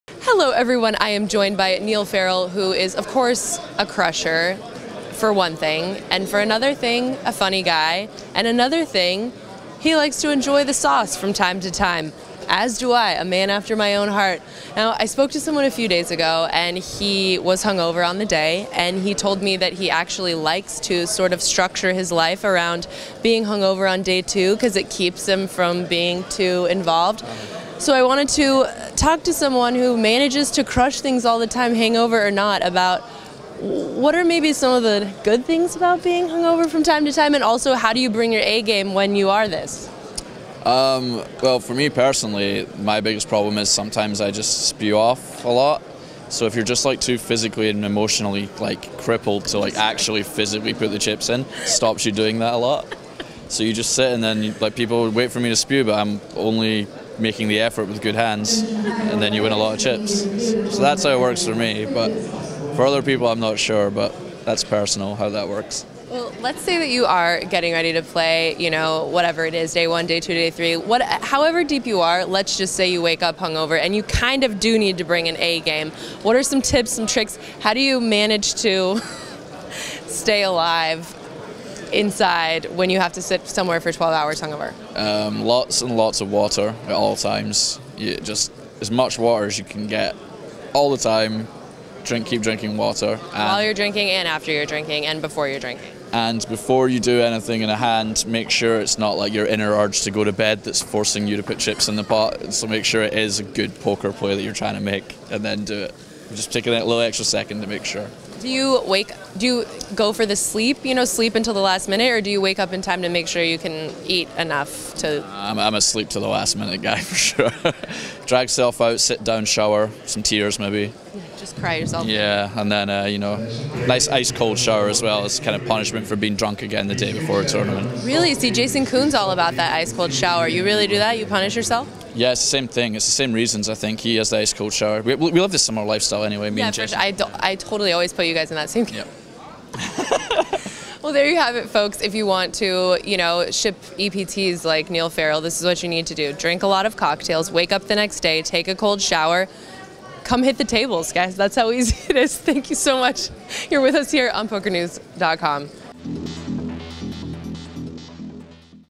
How to Play a Tournament Hungover
After Sarah Herring talks to a player who admits he likes to play hungover early in a tournament, she talks to Niall Farrell about the best way to play with a hangover.